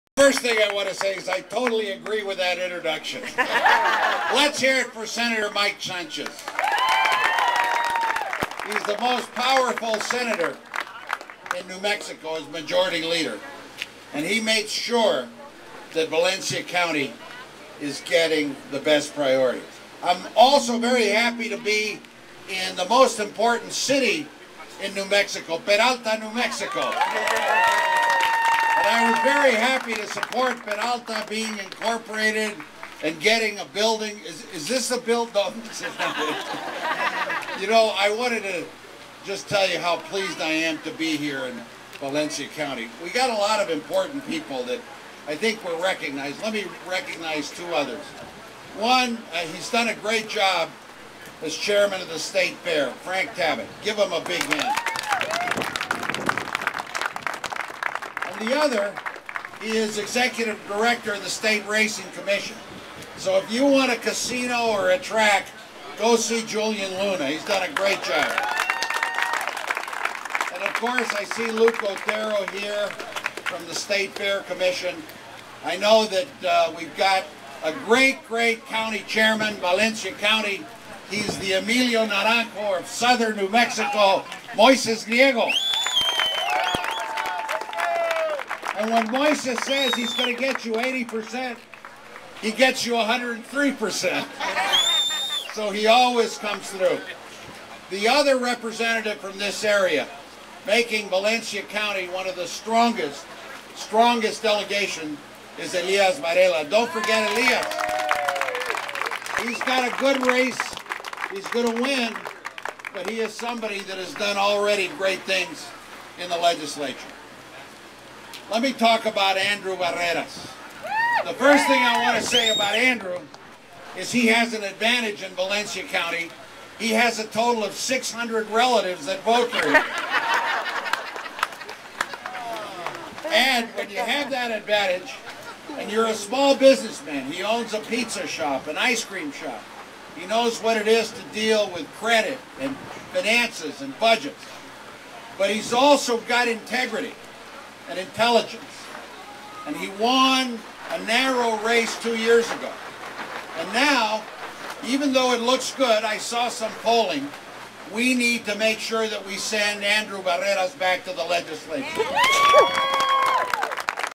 Gov Richardson speaking at Matanza in Valencia County
Matanza for Rep. Andrew Barreras Valencia County